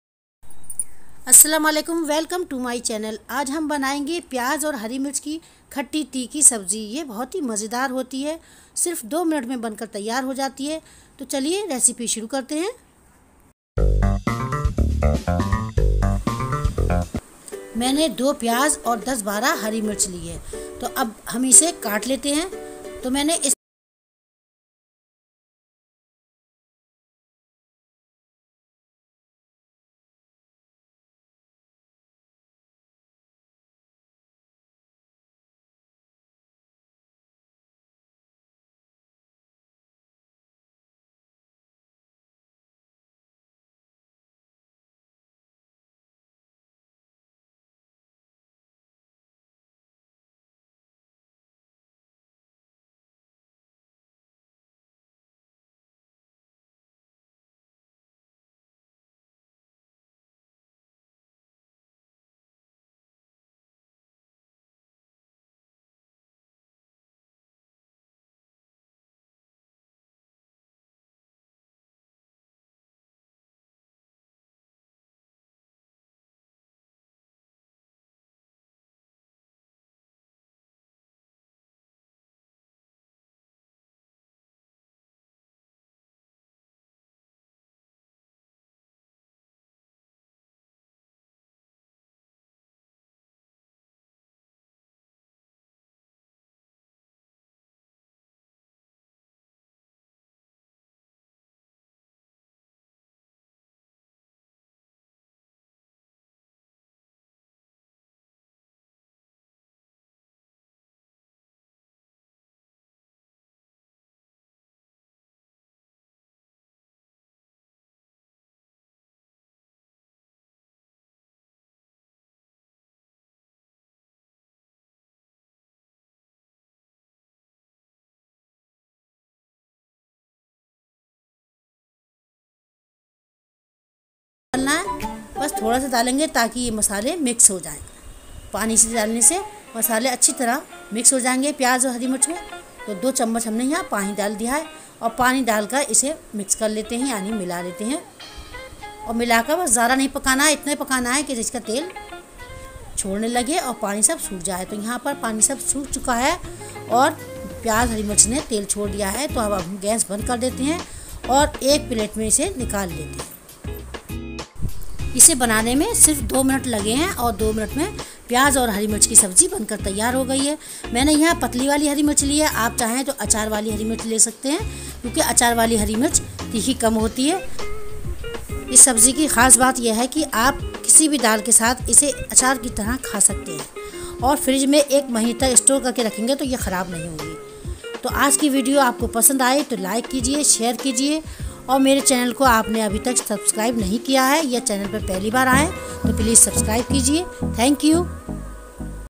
2 min m banne wali pyaaz hari mirch ki sabji | प्याज़ हरी मिर्च सब्जी | Tabassum khan recipe
2 min mei banaye jane wali pyaz or hari mirch sabji jhatpat recipe..